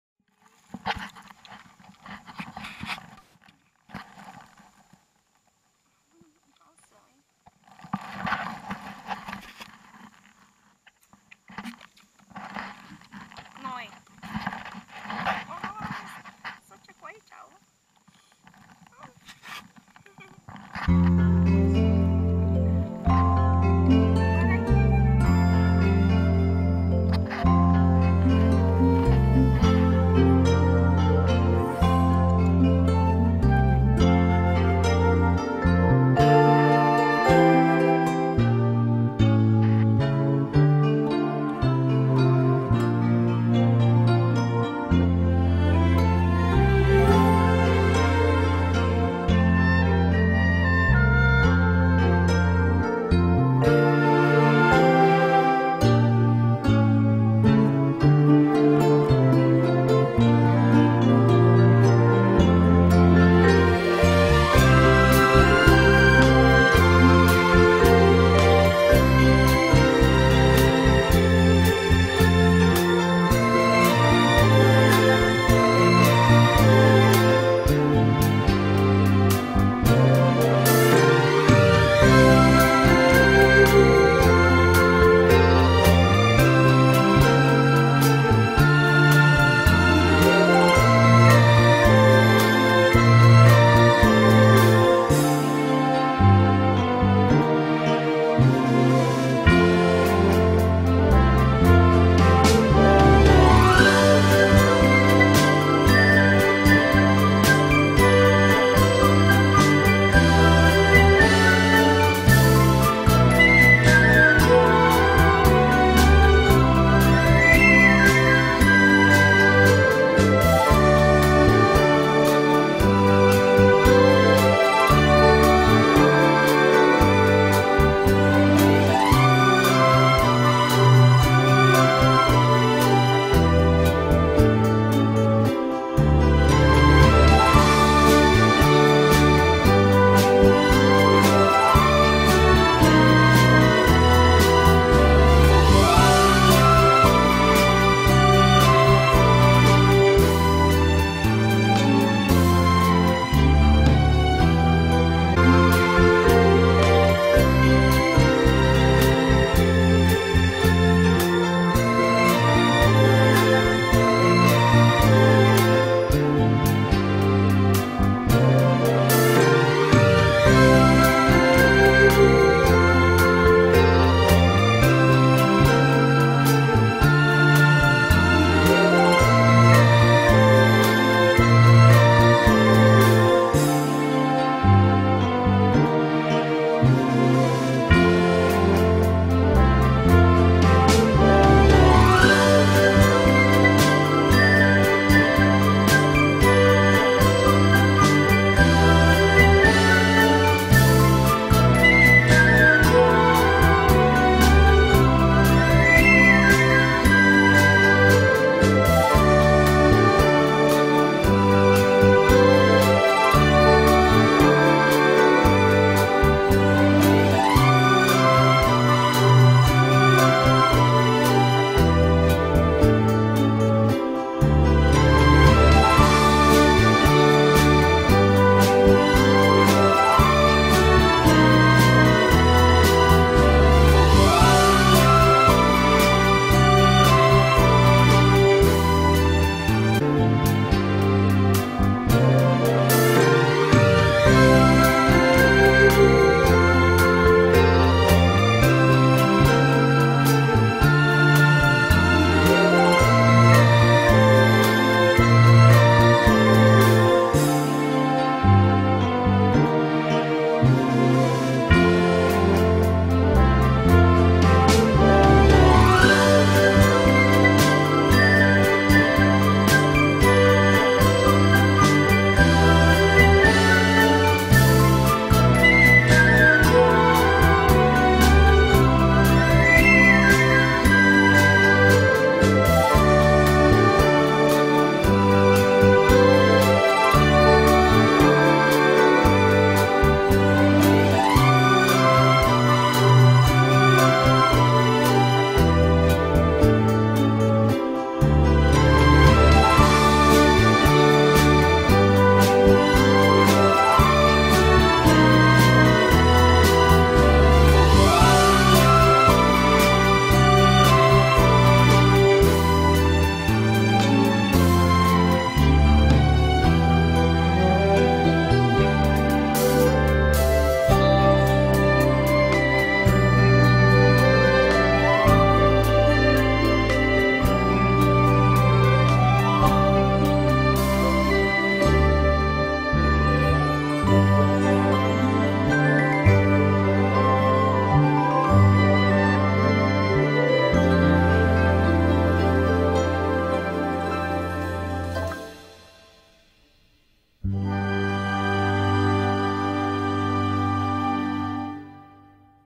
Countryside Hoi An: Part Two
Daytrip through the countryside of Hoi An